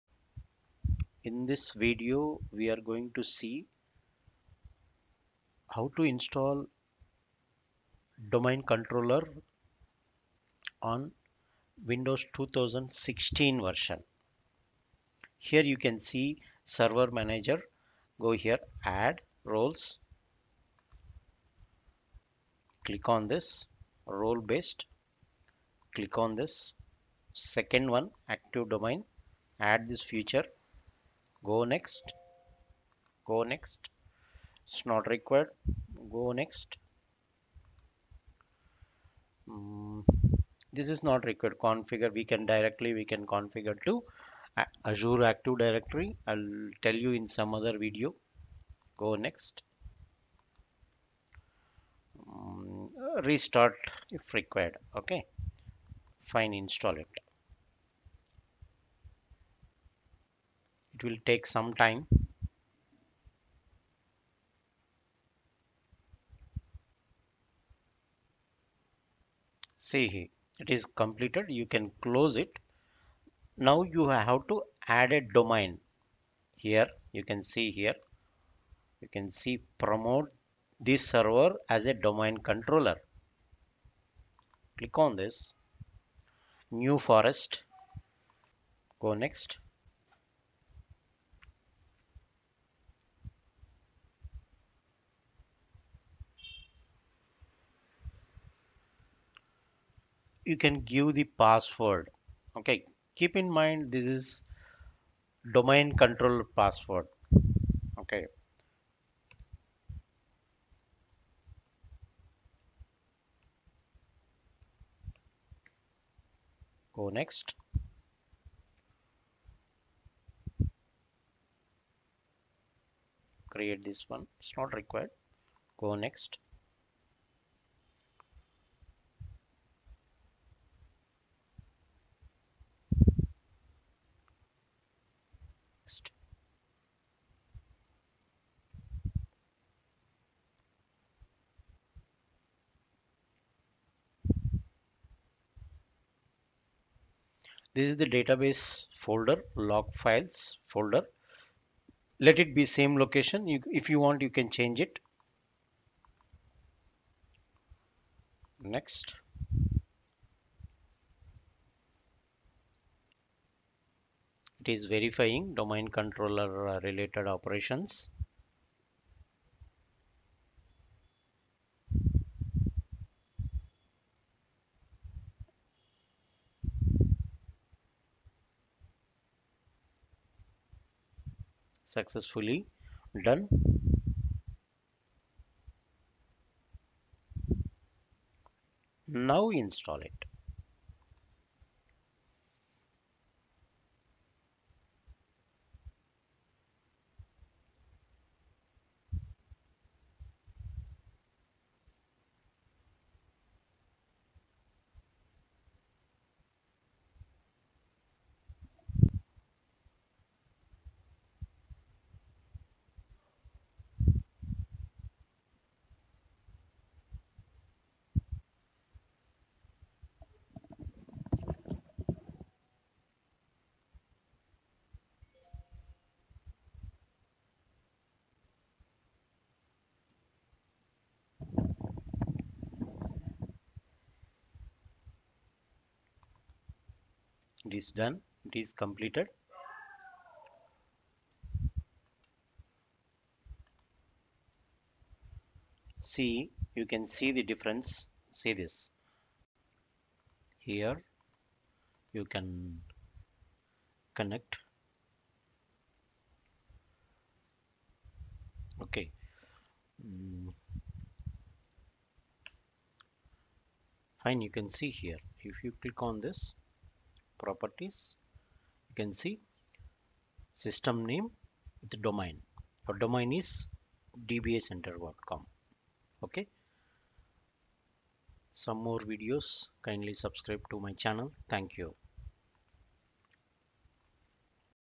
Installing Domain Controller on Windows Server 2016
Installing Domain Controller and configuring a domain dbacentre.com on Windows Server 2016